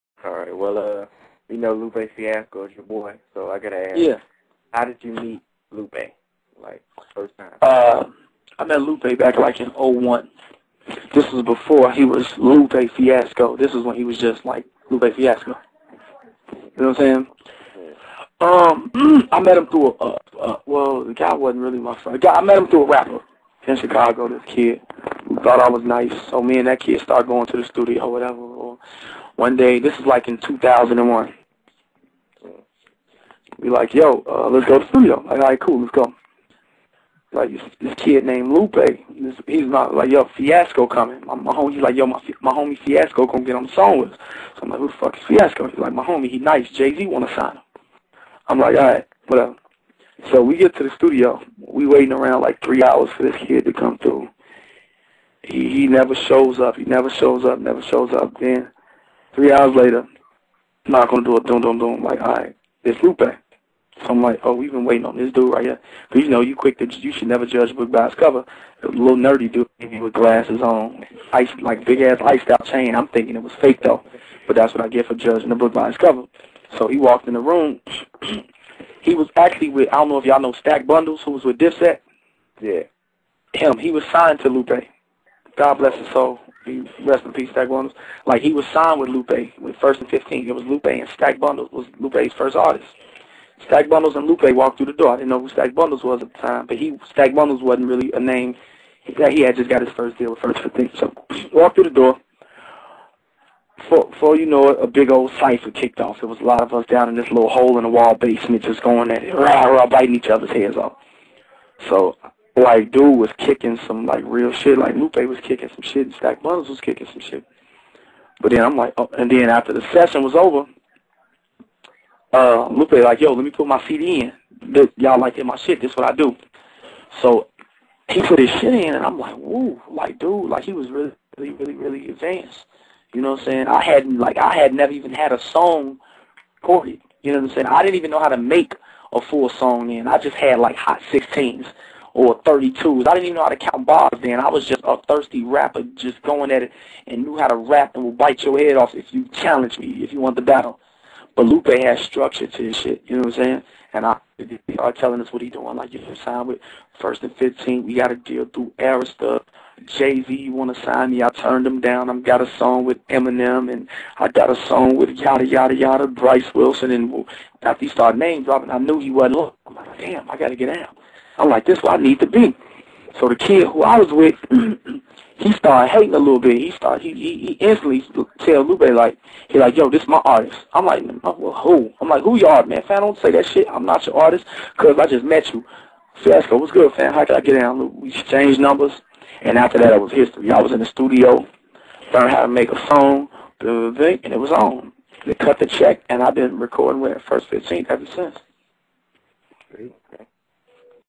How Gemstones Met Lupe Fiasco
The story behind how FNF Artist Gemstones met Lupe Fiasco.